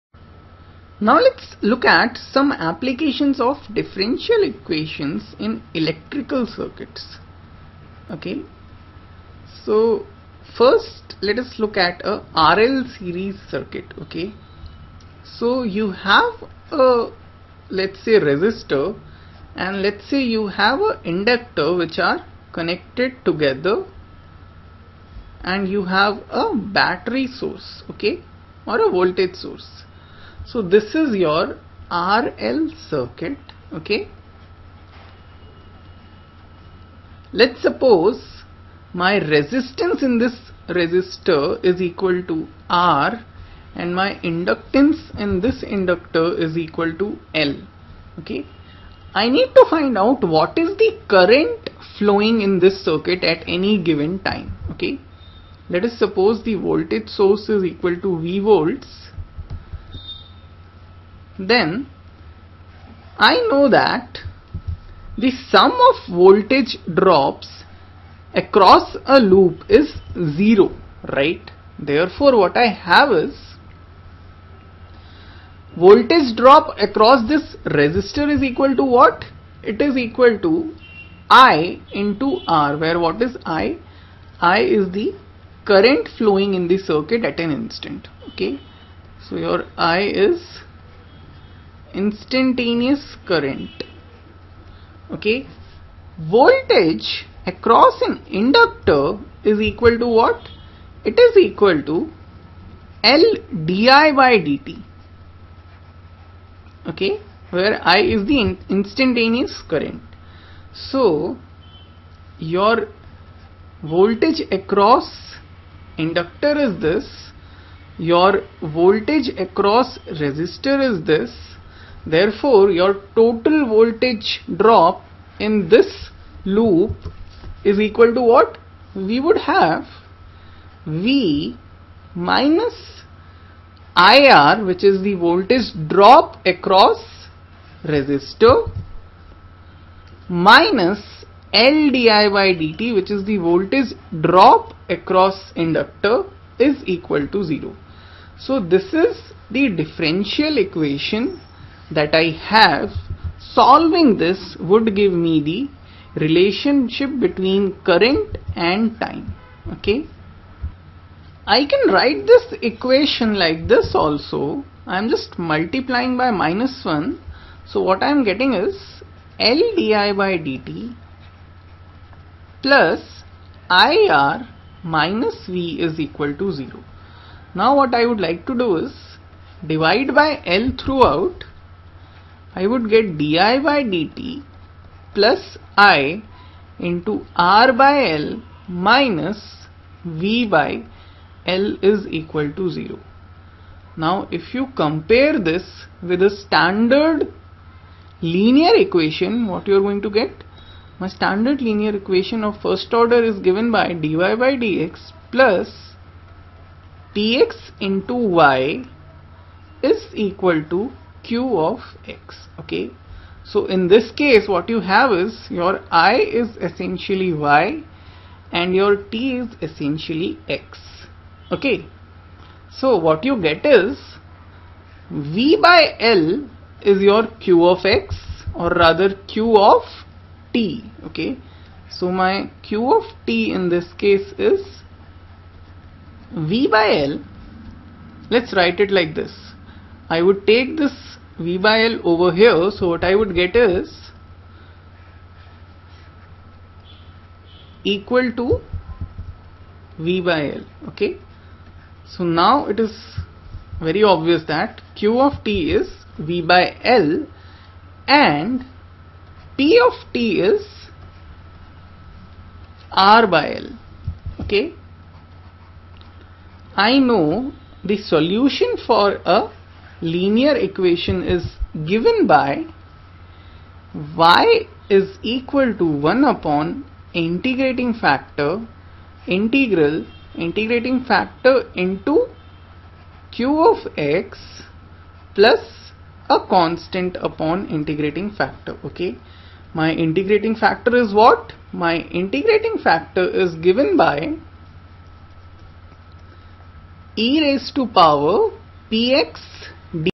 Electrical Circuits and Radioactivity with Differential Equations
Electrical Circuits, LR Series Circuit, RC Series Circuit, First Order RC Circuit, Radioactivity